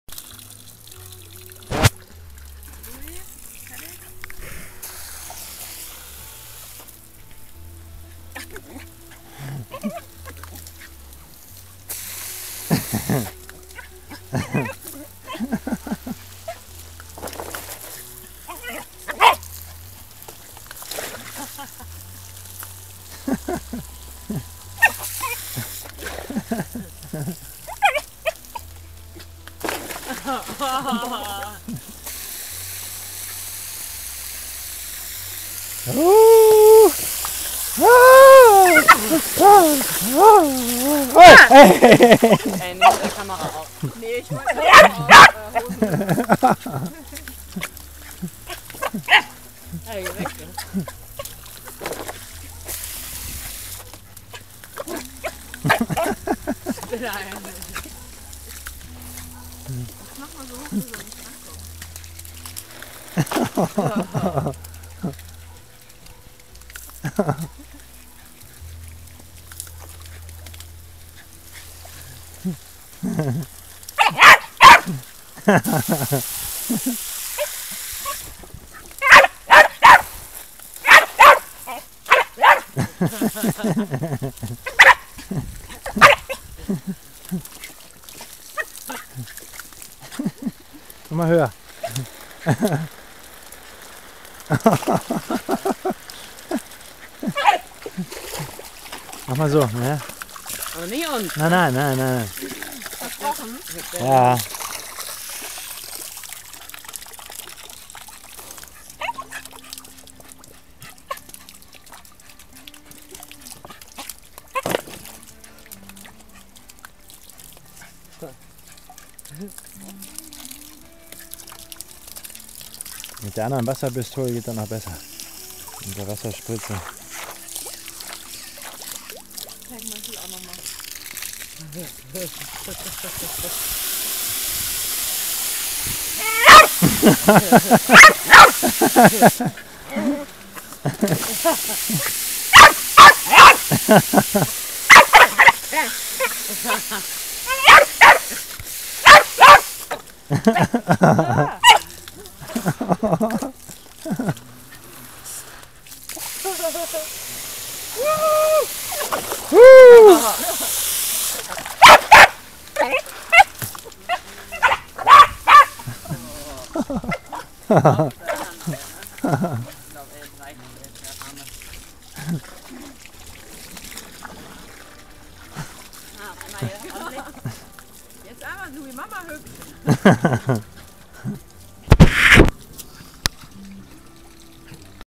Jack Russel in Pool -
Jack Russel plays with water in Pool.

Hund spielt im Pool mit Wasser.